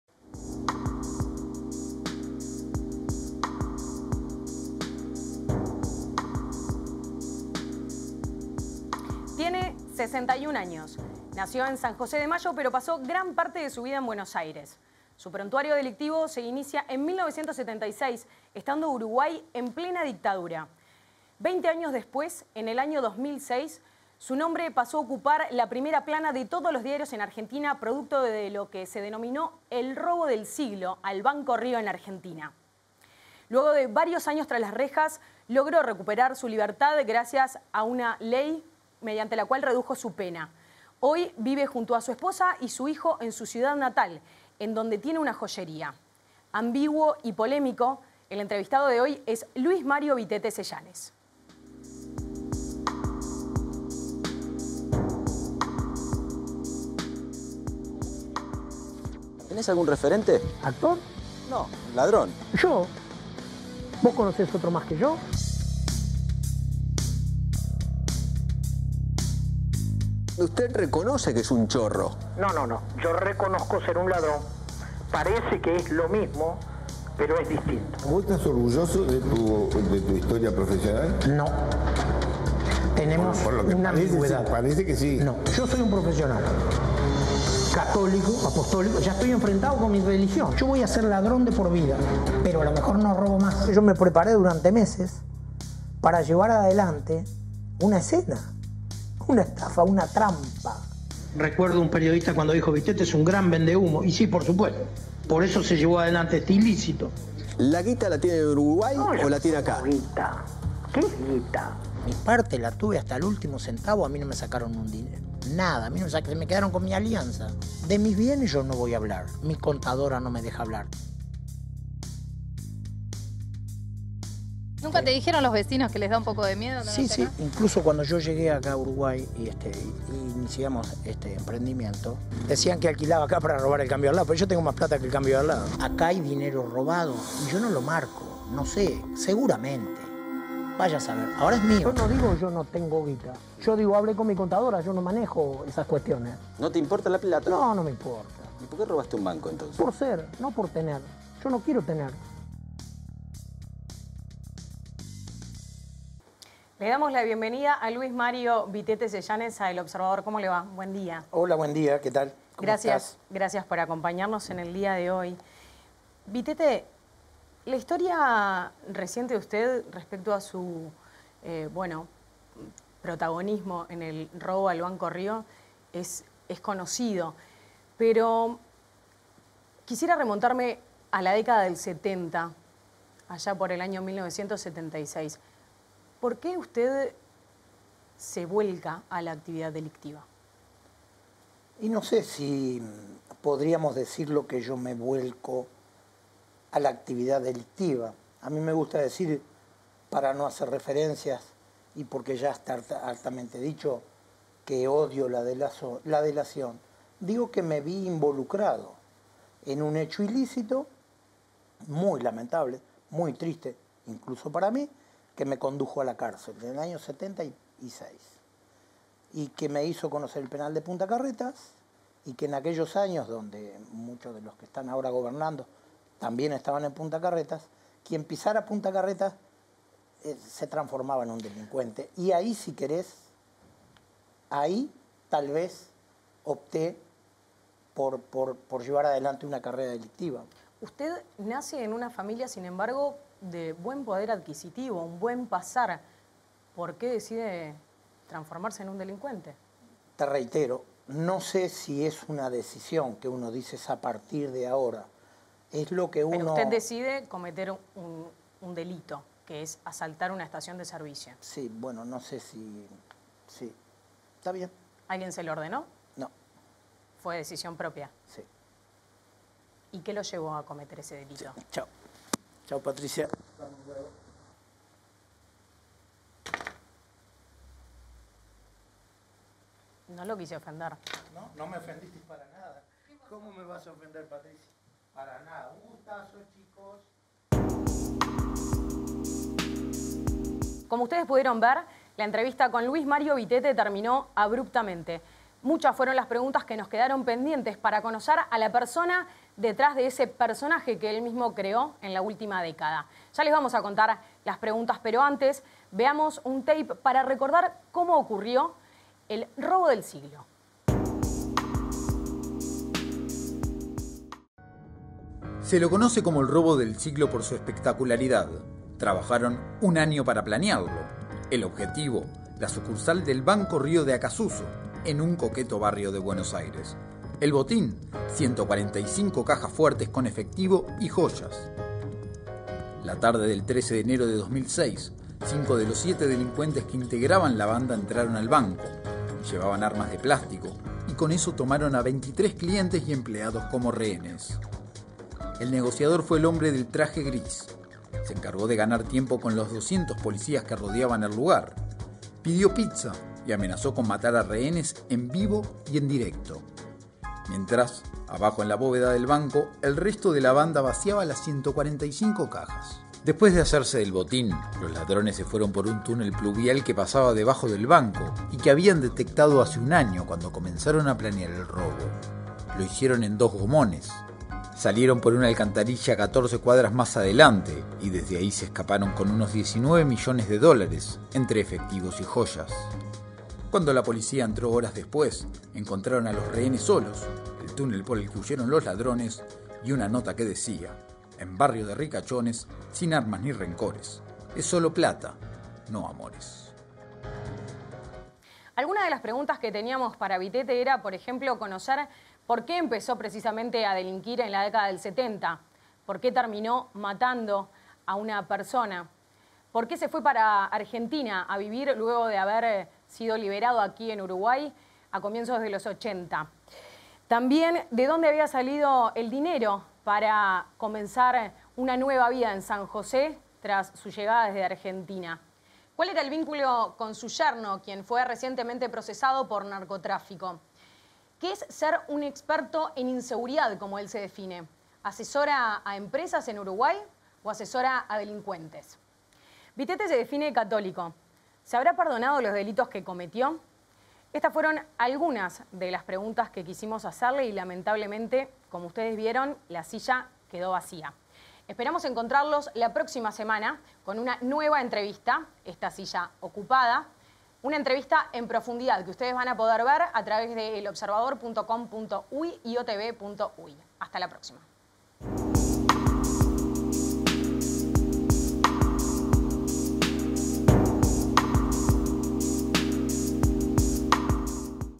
Las preguntas que le molestaron a Mario Vitette y la entrevista que no fue
Mario Vitette Sellanes era un hombre desconocido hasta que en 2006 su nombre ocupó los titulares de todos los diarios argentinos por ser señalado como el cerebro detrás del denominado 'robo del siglo' al banco Río de Acasusso.

Pero su historia delictiva arrancó algunas décadas atrás, en San José. Vitette fue procesado el 28 de Mayo de 1976 por "homicidio y rapiña" en una estación de servicio, según el Juzgado letrado de esa localidad. Su víctima fue un sereno que trabajaba en el comercio.

Al ser consultado sobre este hecho en el ciclo de entrevistas que El Observador TV estrenó con la conducción de la periodista Patricia Madrid, Vitette Sellanes abandonó el estudio.